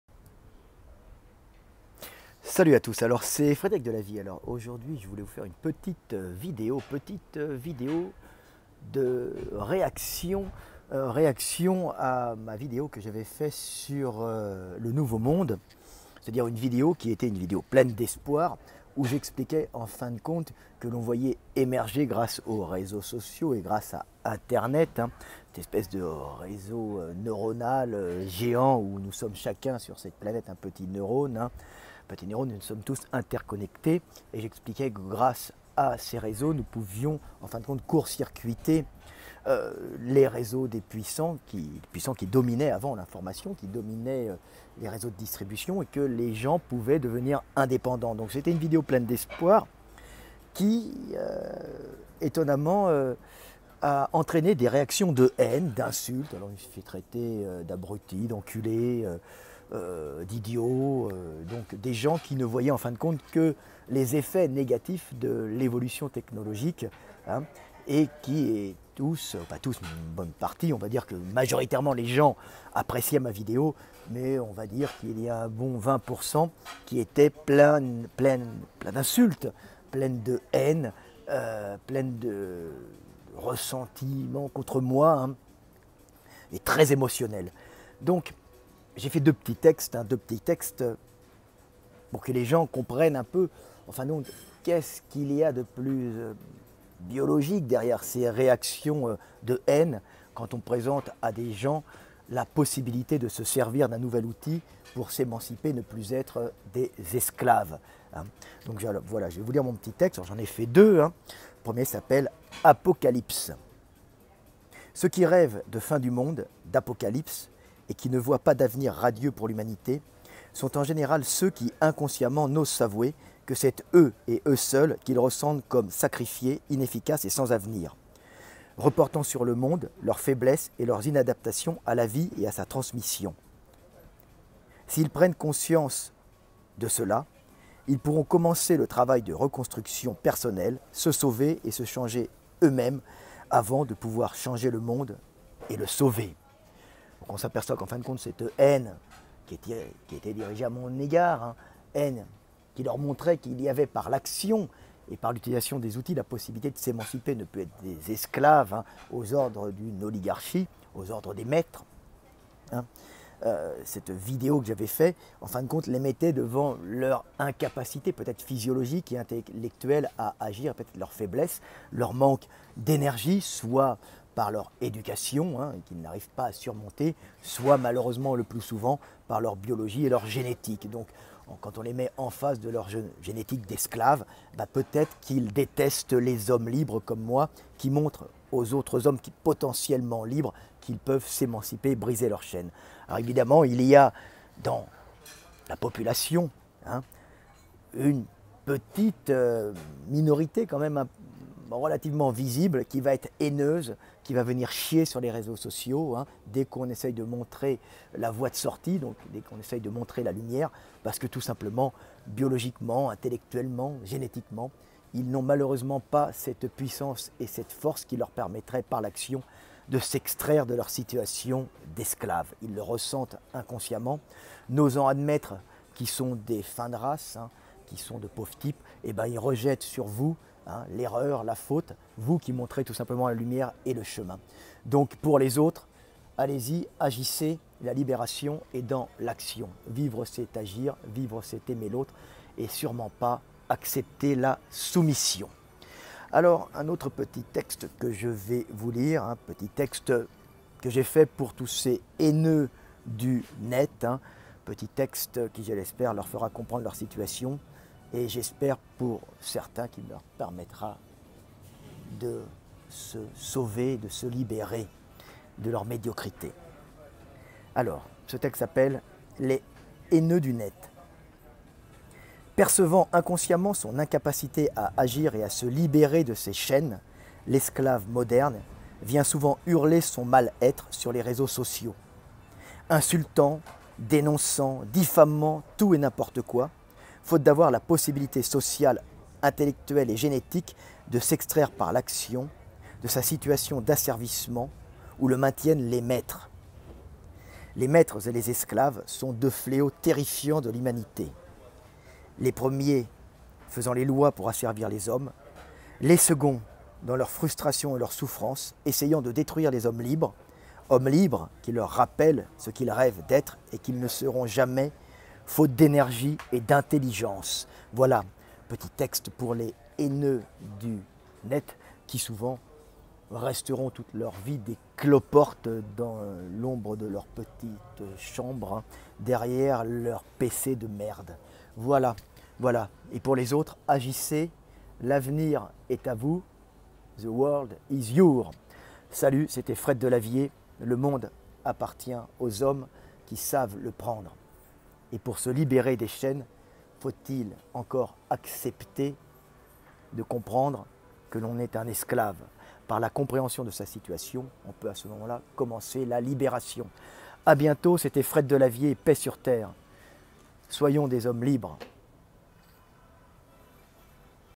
Les haineux nihilistes du net.
Frédéric Delavier apprend aux haineux du net à se libérer de leurs chaînes pour devenir des hommes libres.